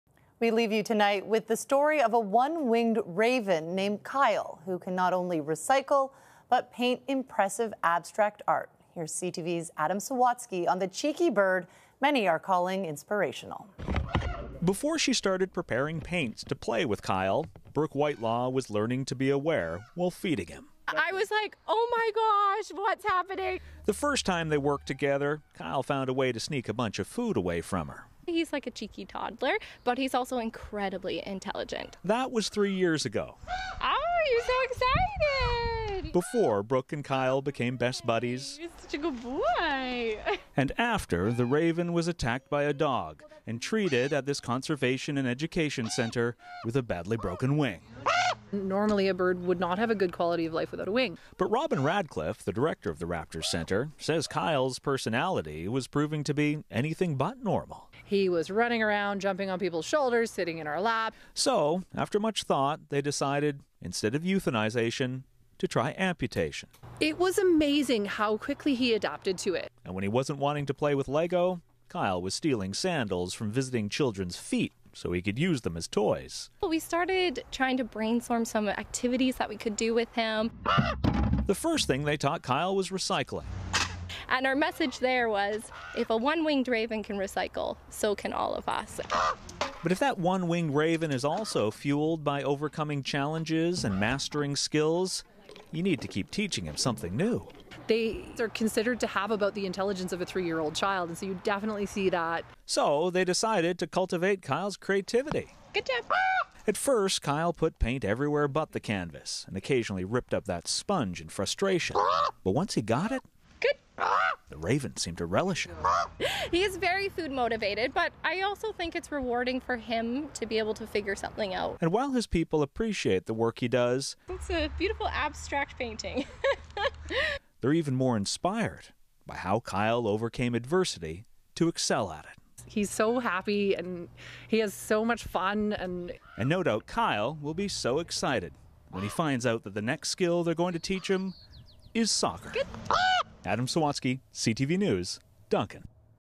One-winged raven develops a passion for painting in British Columbia
After an incident with a dog left Kyle the raven with one wing, he would later develop a passion for painting.